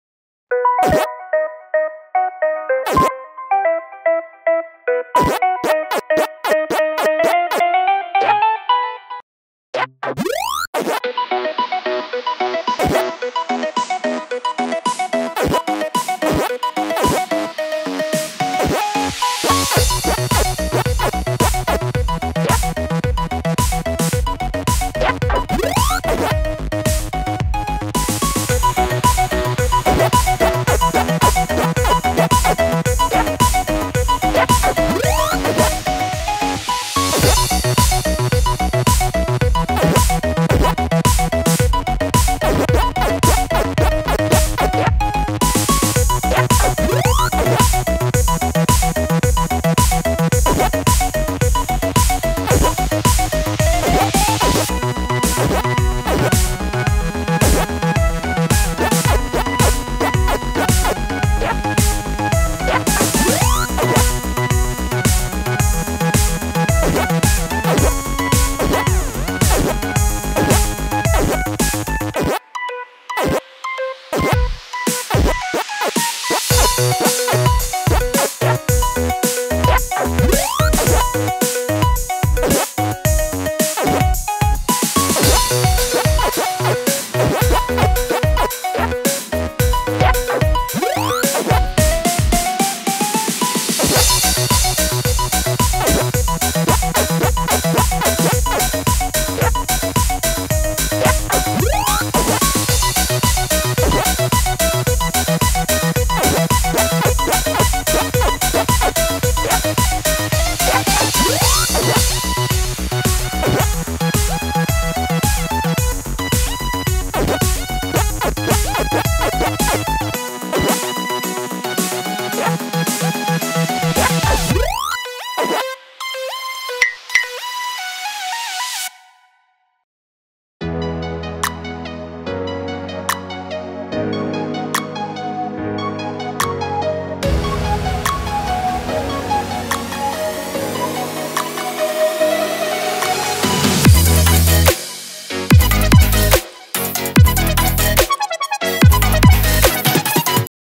Pacman vs Funny Dentures
Pacman finds all kinds of funny dentures and tries them all.